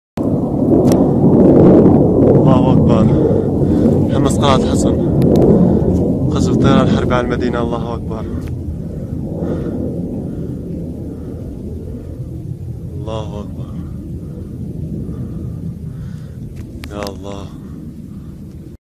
شام حمص قلعة الحصن تصاعد الدخان جراء القصف على المدينة 16 2 2014
مراسل شبكة شام أبو مروان الحصني يرصد تصاعد الدخان جراء القصف على مدينة قلعة الحصن في ريف حمص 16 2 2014